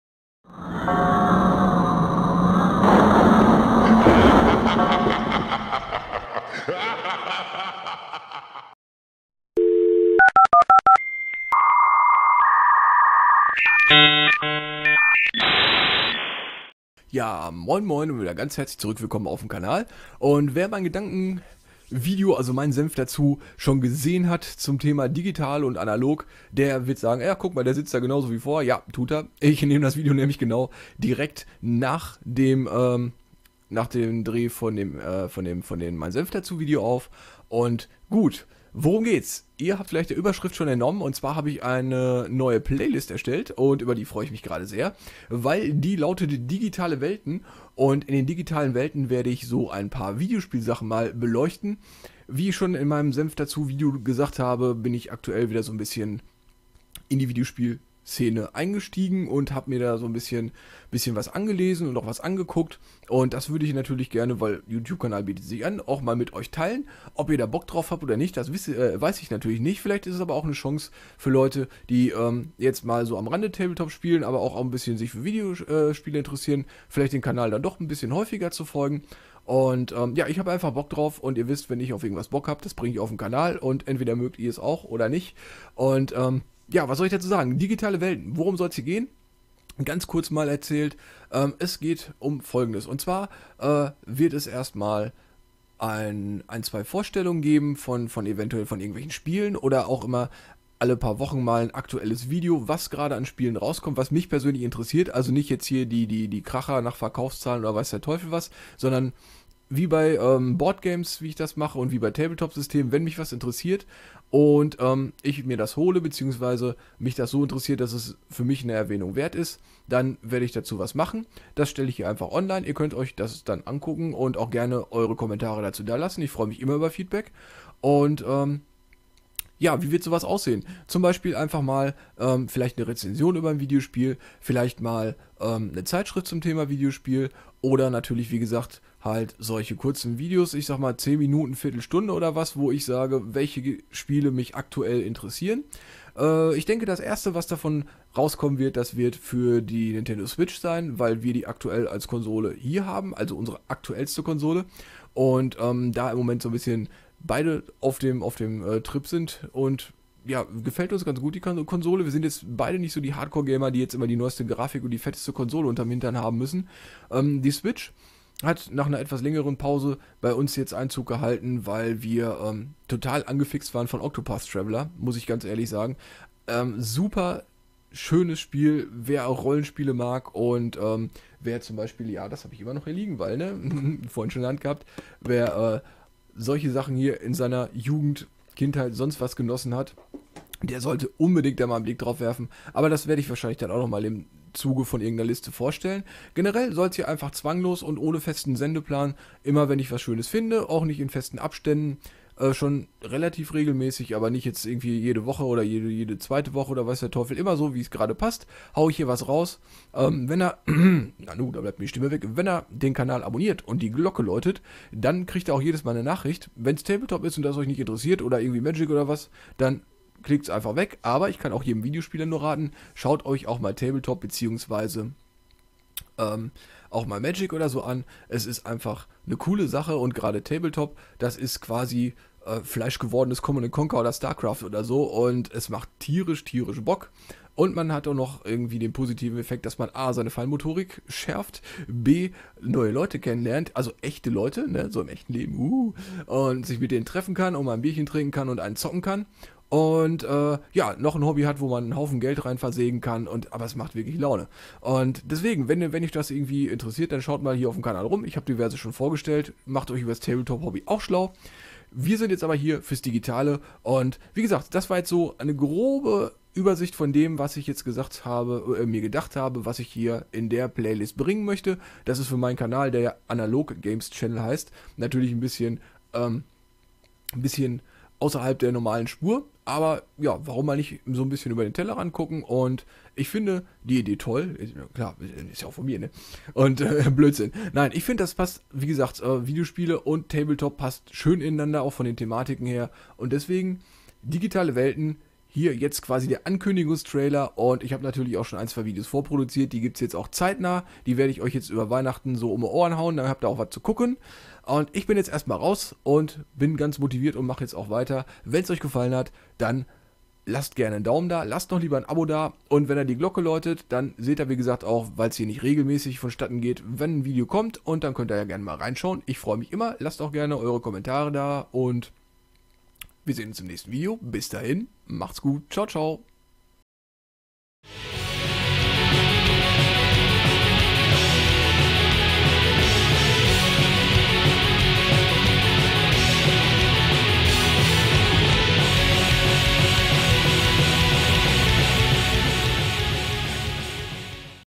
ANKÜNDIGUNG: Digitale Welten - Die neue Playlist !
Moin, Moin,
jaaaa, der Eine oder Andere hat es sich vielleicht schon gedacht, dass, da ich ja aktuelle meine alte Videospiel-Liebe wieder entdeckt habe, auch hier hier auf dem Kanal evtl. das ein oder andere zum Thema zu sehen sein wird ;-)
Also nicht lange gefackelt, here it comes ;-)
Viel Spaß in der etwas anderen Playlist hier auf dem Kanal :-)